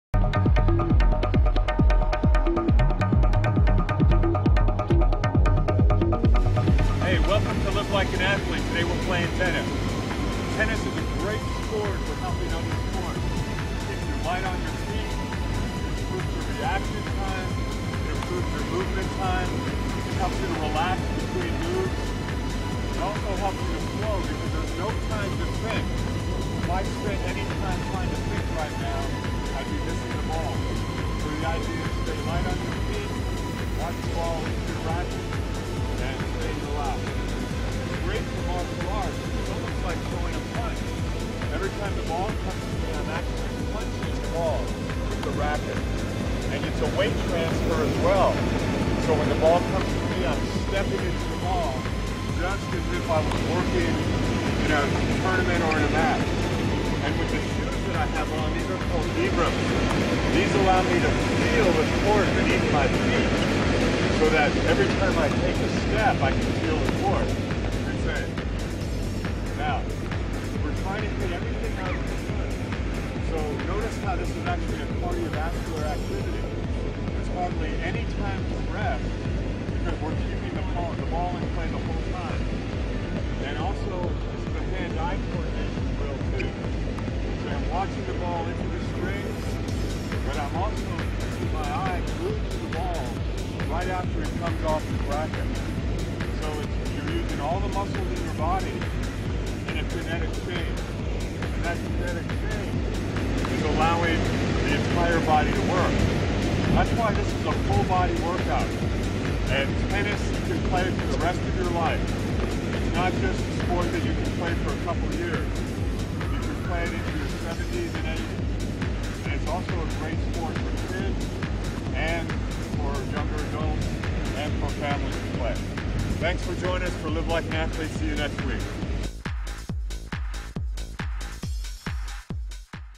Put Tennis In Your Toolbox
Tennis is a great sport that appeals all ages. Dr. Tom Seabourne of NTCC gives an overview over how tennis is beneficial to one's active lifestyle.